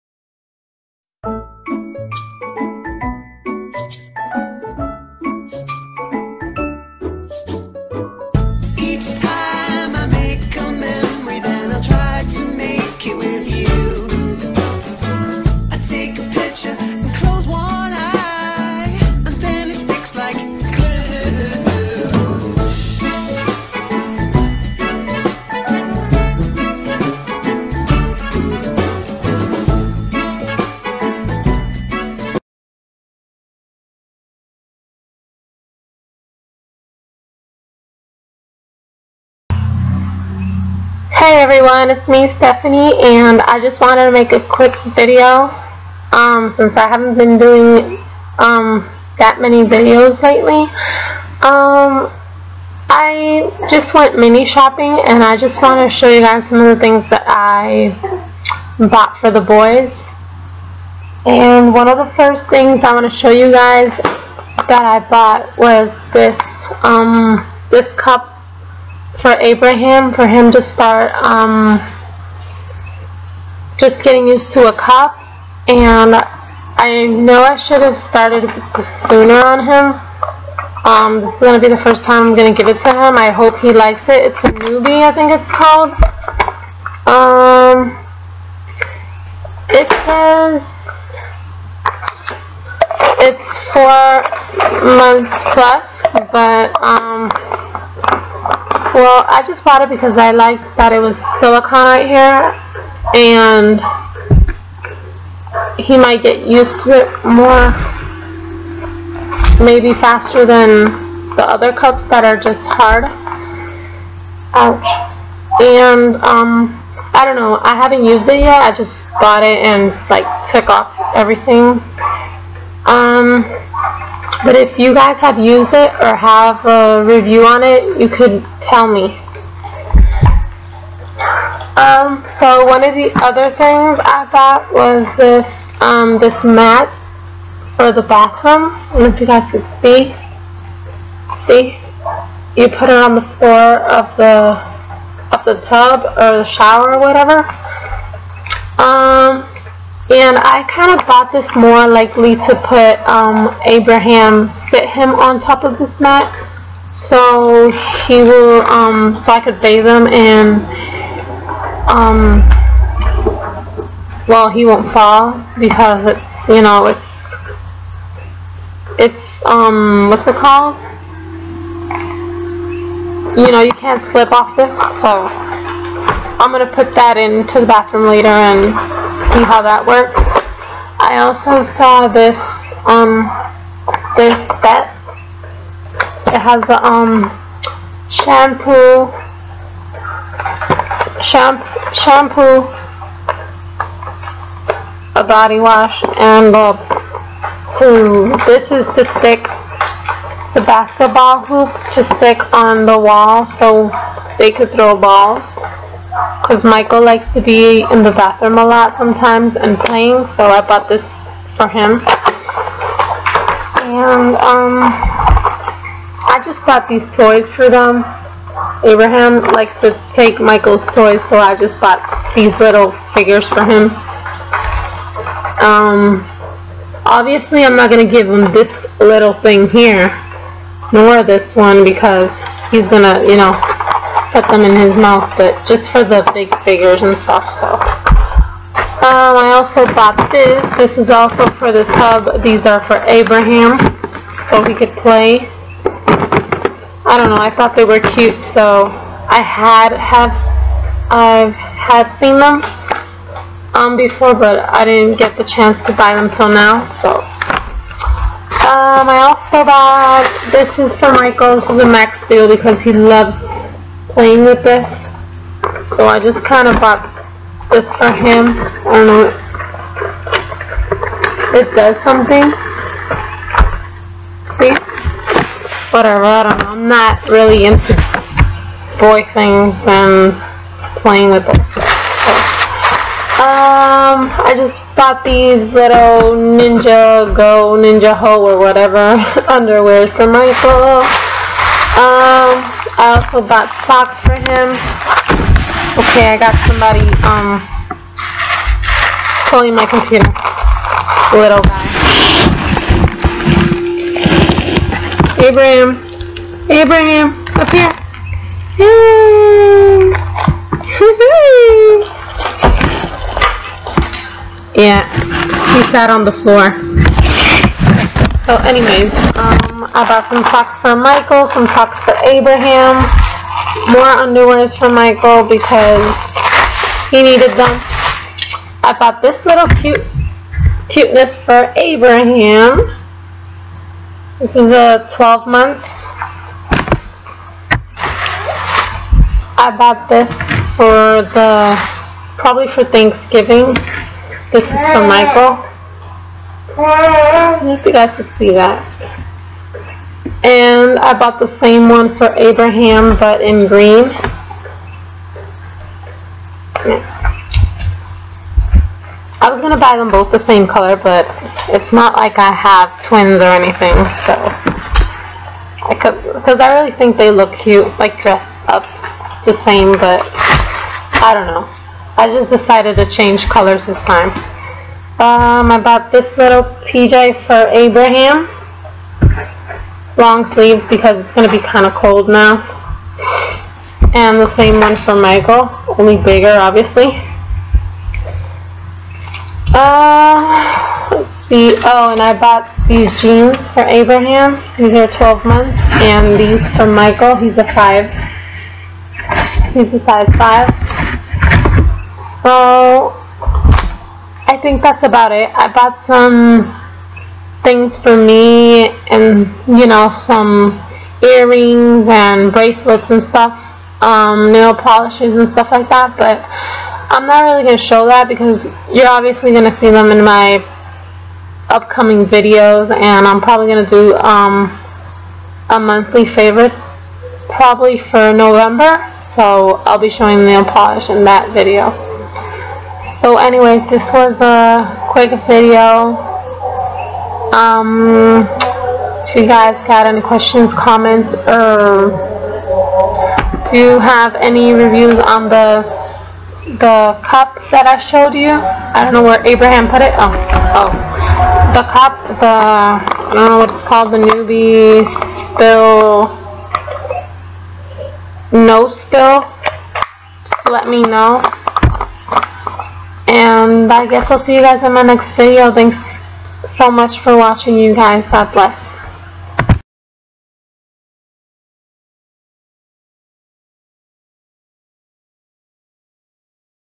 Mini Haul: The Boys
I thought I might share some things I  bought today for the boys.